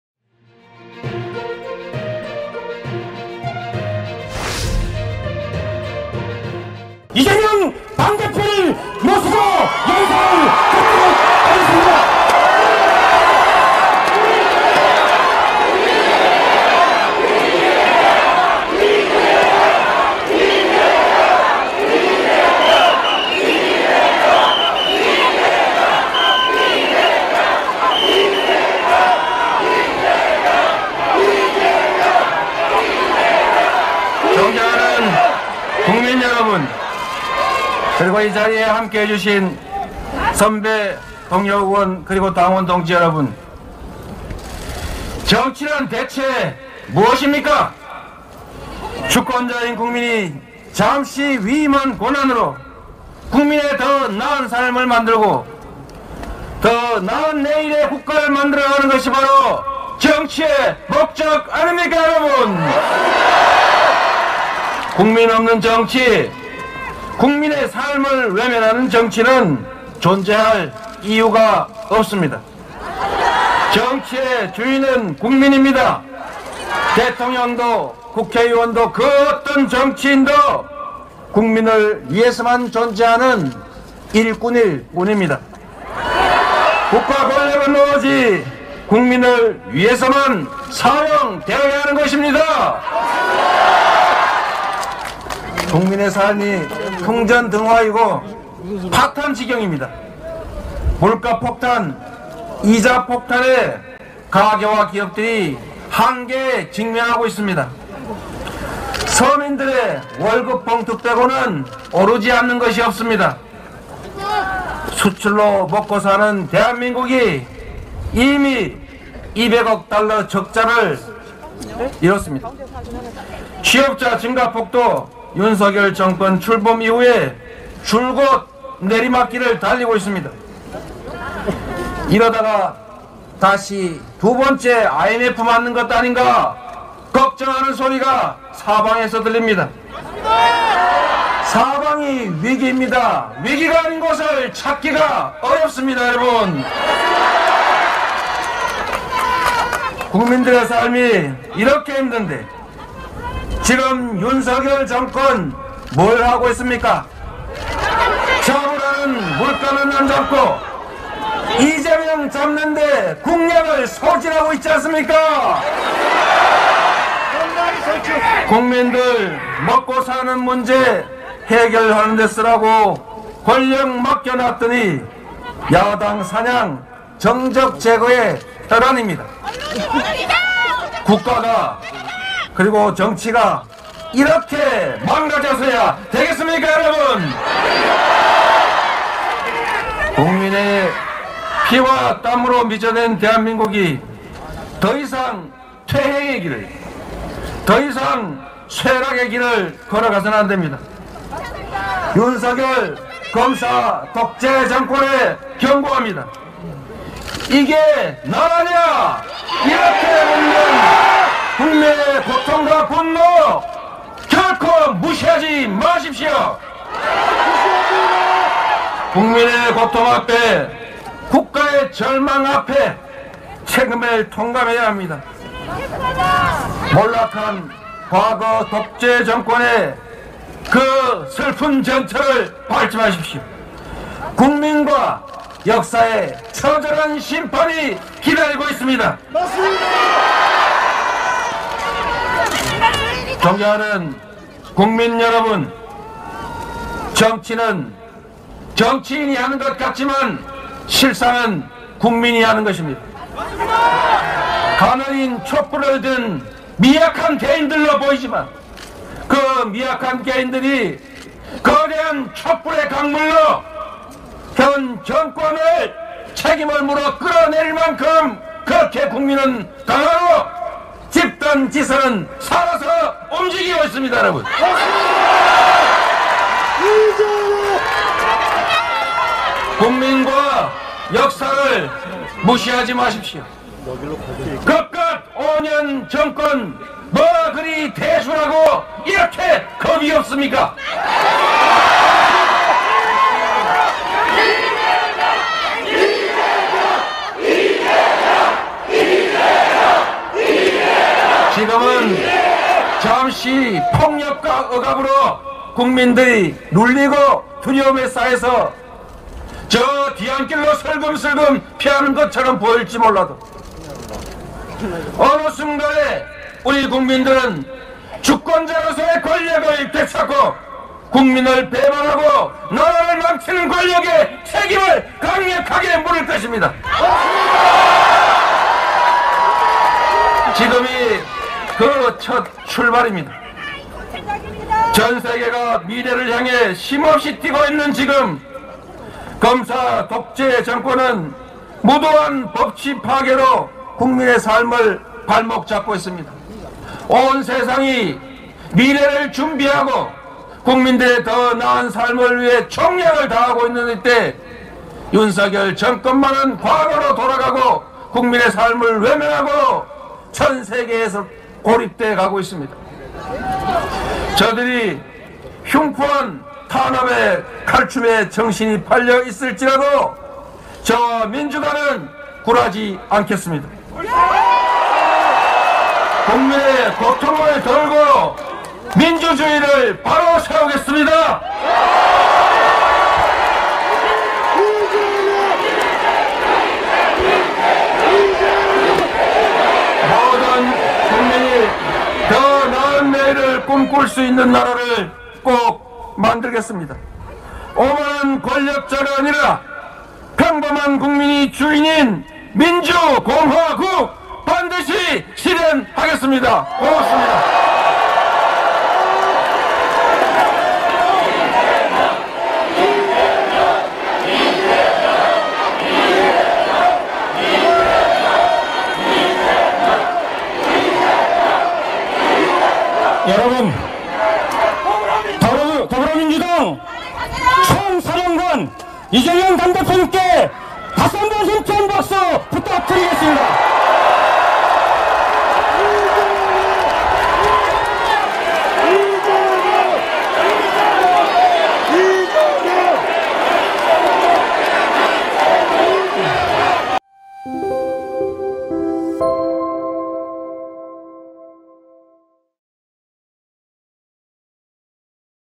‘이게 나라냐’ 외친 이재명 “윤석열 검사독재 칼춤에 굴하지 않겠다”
2023년 2월 17일 국회본청 계단 앞에서
더불어민주당 윤석열 정권 검사독재 규탄대회가 진행되었다.